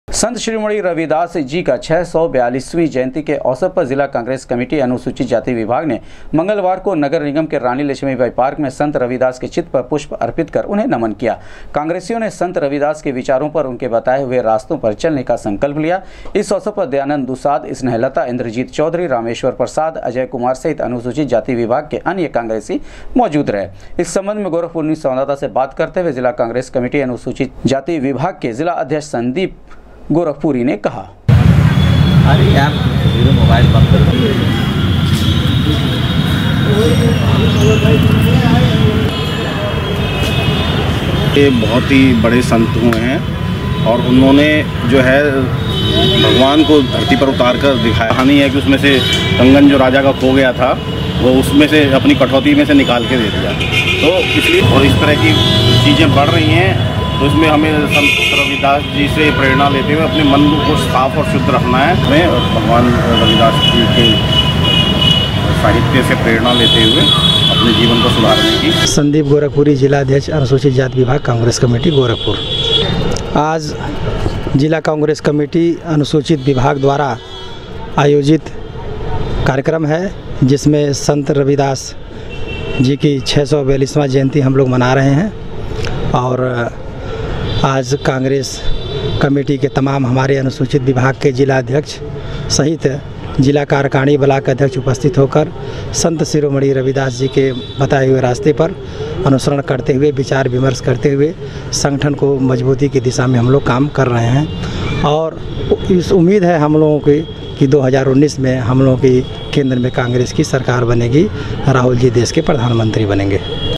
जिला कांग्रेस कमेटी अनुसूचित जाति विभाग ने, पुष्प अर्पित कर मनाया, संत शिरोमणि रविदास की
जिला कांग्रेस कमेटी अनुसूचित जाति विभाग ने, पुष्प अर्पित कर मनाया, संत शिरोमणि रविदास की 642 वीं जयंती, संत रविदास के विचारों पर चलने का लिया संकल्प।